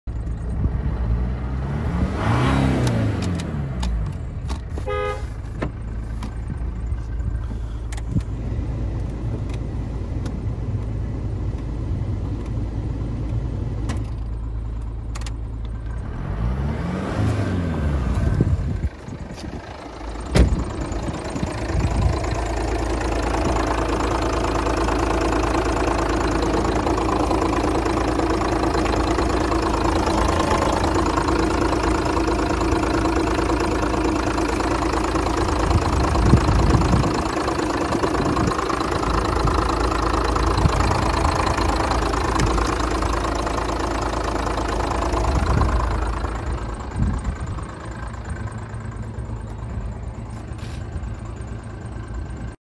Разборка Jeep - Liberty 2002-2006, 2.5 Турбо (VIN: 1J4GMN8792W195690) YR02UWX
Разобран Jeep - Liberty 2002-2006
Основные характеристики:
Год выпуска: 2002
Двигатель: 2W195690
Объём двигателя: 2.5
Тип двигателя: Дизель
Коробка передач: КПП 5-ст.мех 4х4 (МКПП)
Кузов: Джип (5-дверный)
Смотри каталог запчастей Jeep - Liberty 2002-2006 на :
⏰ Режим работы колл-центра:
08:00 – 20:00 – ежедневно

Для наших покупателей:
- бесплатная доставка;